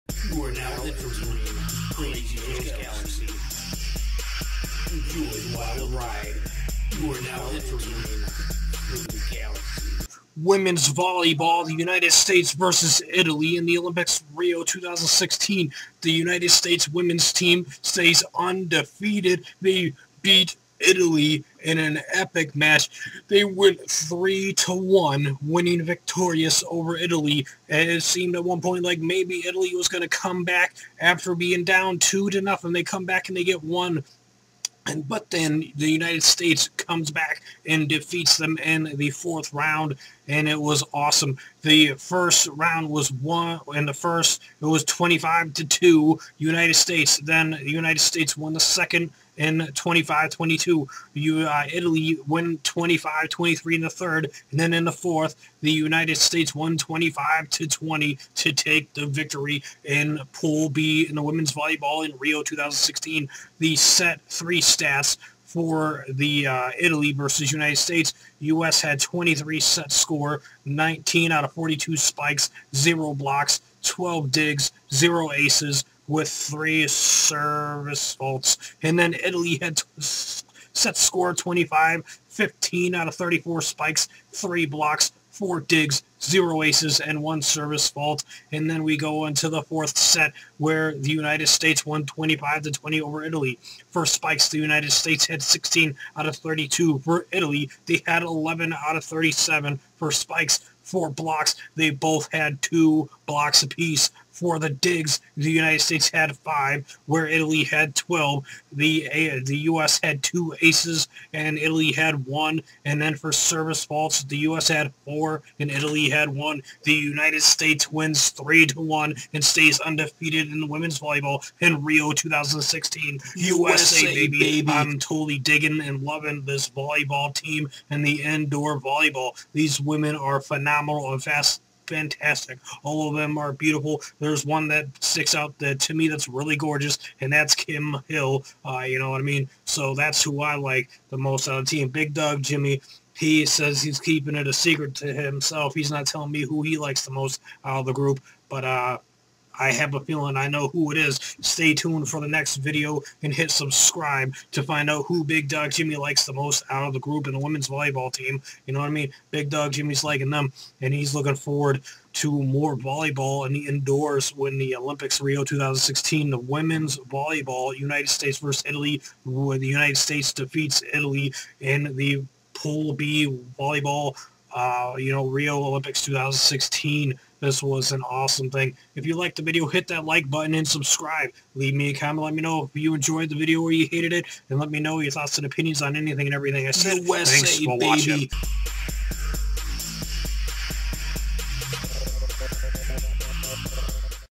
Women's VolleyBall USA VS ITALY: Rio 2016 Olympic Indoor Volleyball Review
Review & Results of USAVwnt VS ITALY Women's Volleyball Pool B Preliminary Stage of the Women's Volleyball Tournament at the Rio 2016 Olympic Review Italy vs U.S. Women's National Volleyball Team Results